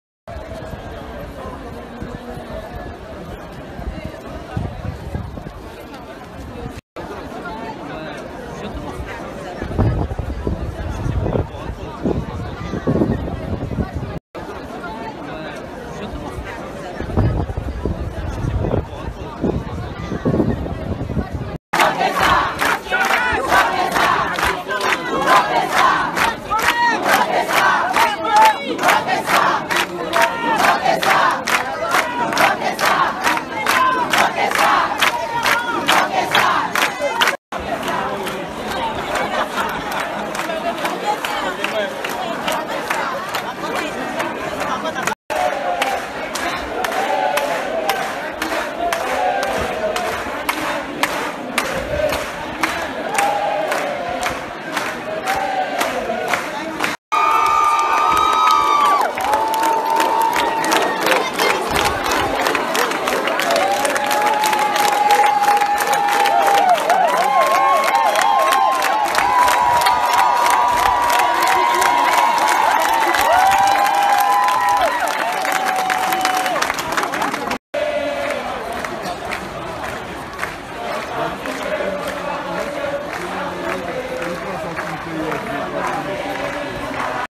'Unprecedented' Protests Sweep French Guiana on Second Day of General Strike
Courtesy: Jean-philippe ROSSI